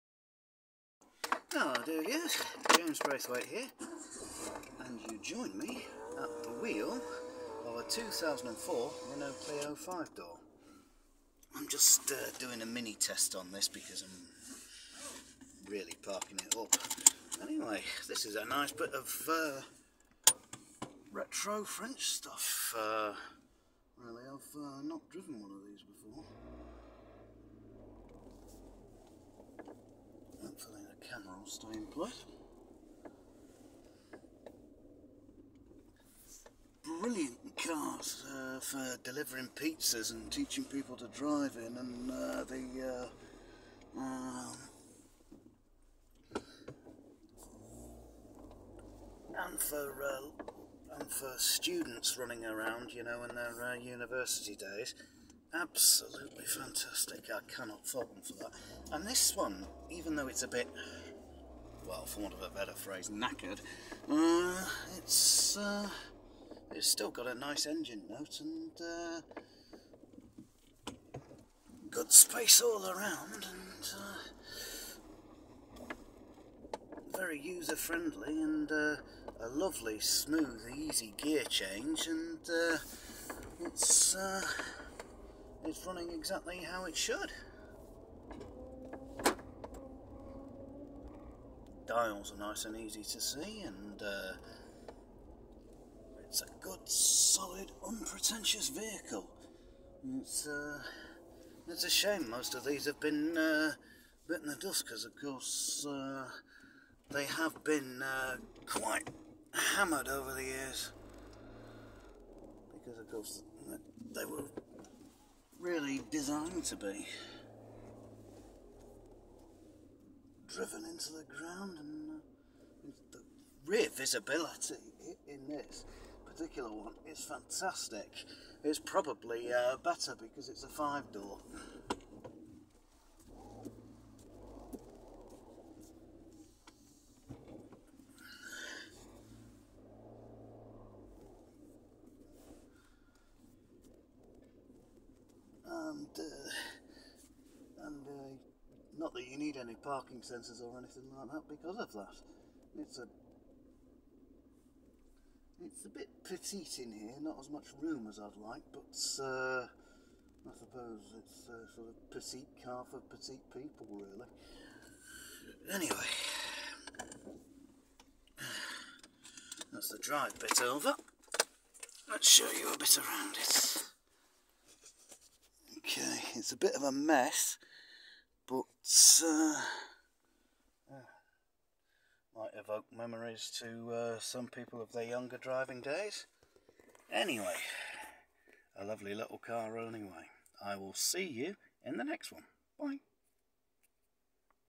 James briefly testing a 2004 Renault Clio 5-dr
I remember when these first came out in 1998. They were a huge hit. They deserved to be. I also remember watching the Clio's racing at Oulton Park 22 or so years back.
This is the first chance I've had to drive one of these Clio's in all that time. As a side note, many people were likely taught to drive in one.
It's all you'd expect from a French city car. Very zippy, unpretentious, easy to handle & this particular example has a wonderful note from the 1.2 litre 16V engine. The steering & gear change is spot on - just the right amount of sensitivity through the wheel & a smooth and almost snicky gear change to boot. I can see why so many people fell in love with them.
I think you may struggle in one if you're over 6' 2", but otherwise, I'd say grab one if you want something rather fun, cheap to insure and easy to use & fix. There aren't many of these left now, unfortunately.